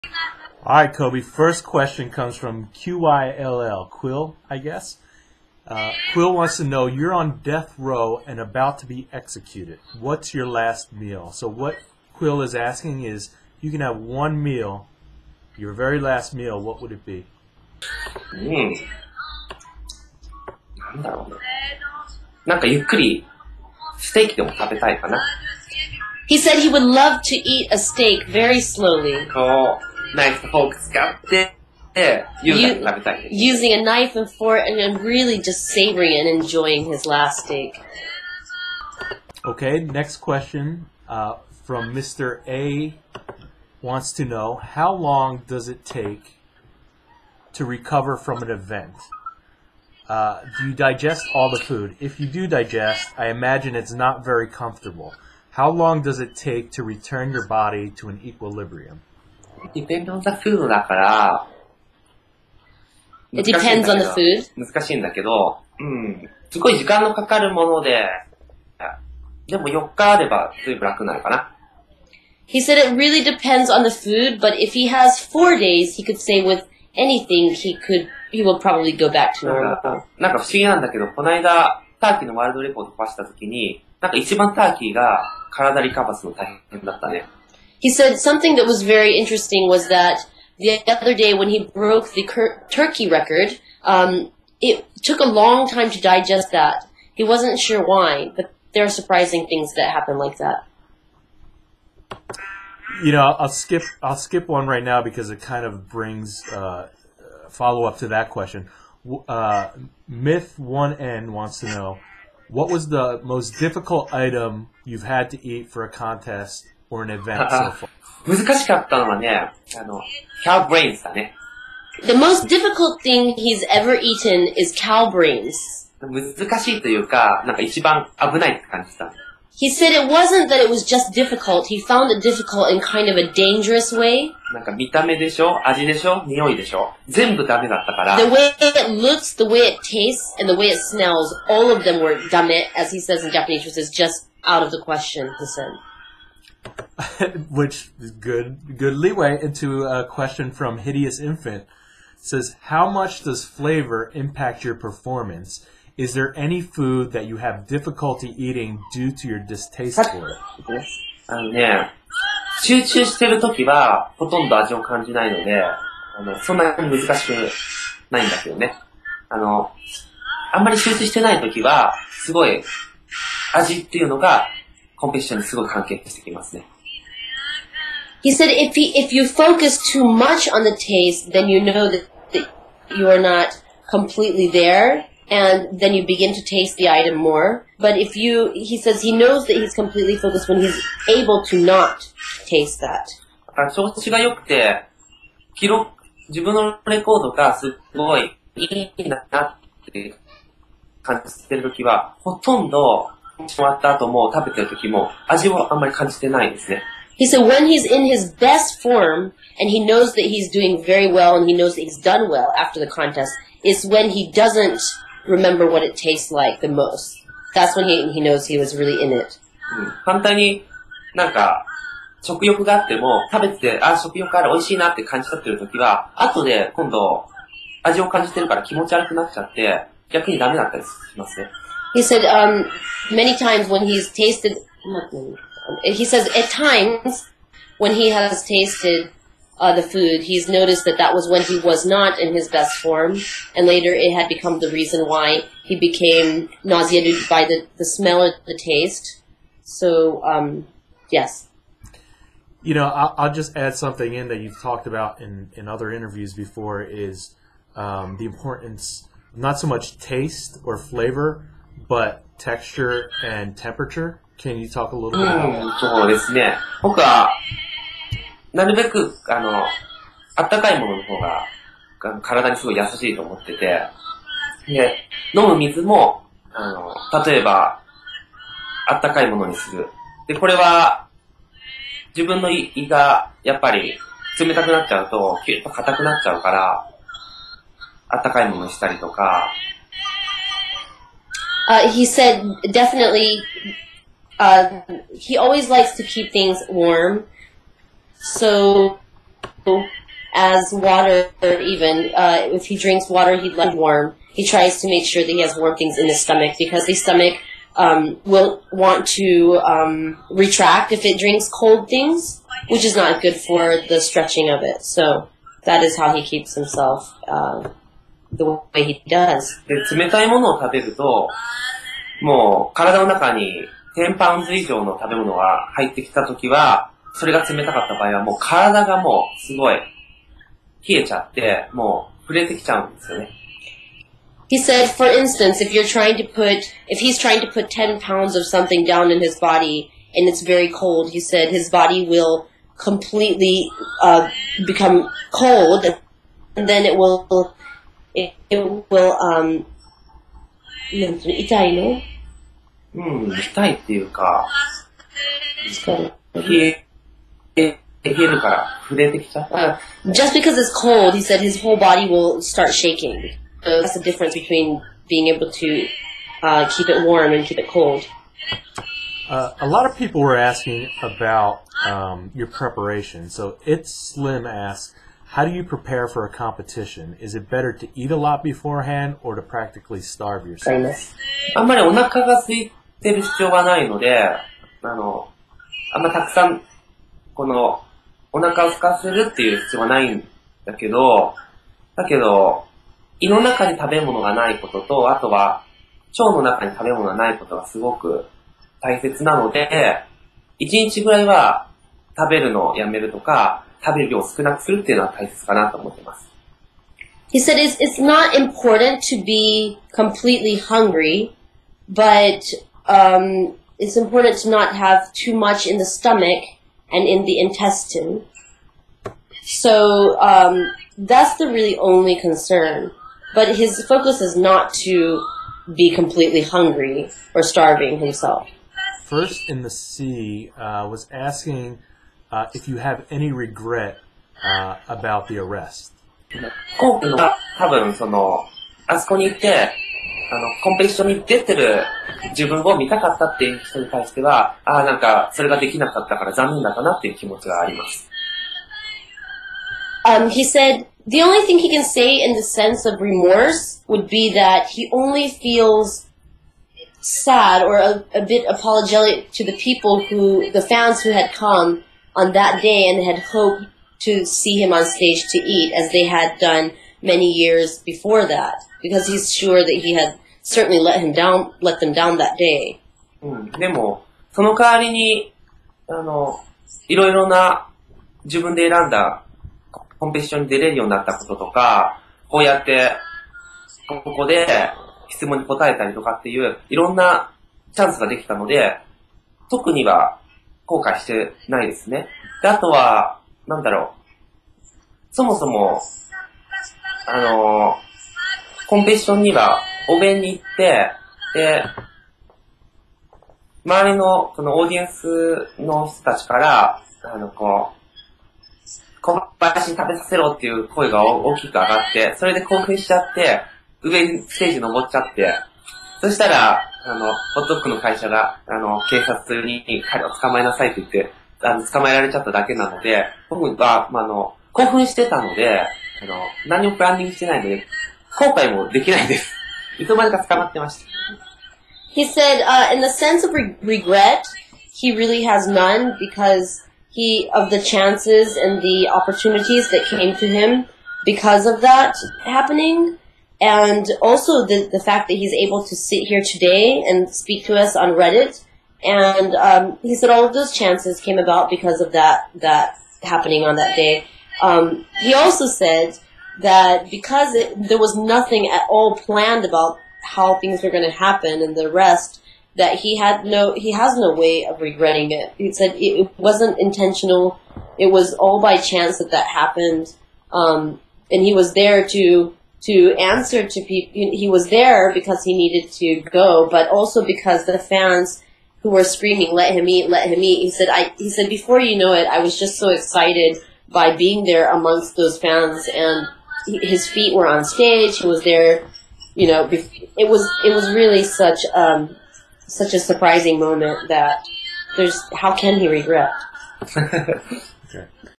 Kobi Reddit AMA Part1
Kobi answers the first series of Reddit questions from fans, including the most difficult foods to eat, his preparation and his recovery.